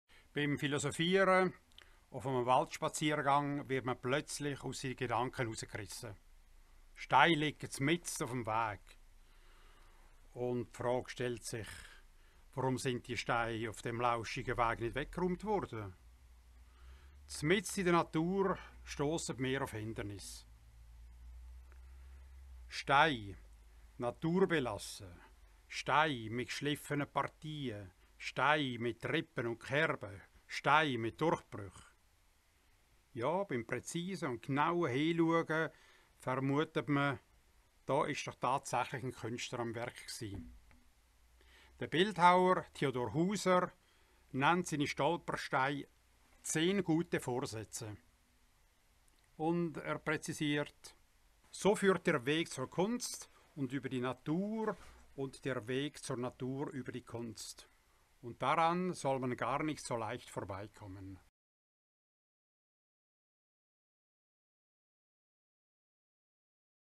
10 gute Vorsätze
Kunstwerk von Theodor Huser auf dem Kulturweg Limmat mit einem herzlichen Dankeschön an Ruedi Sommerhalder für die Erlaubnis, den von ihm gesprochenen Text verwenden zu dürfen (Quelle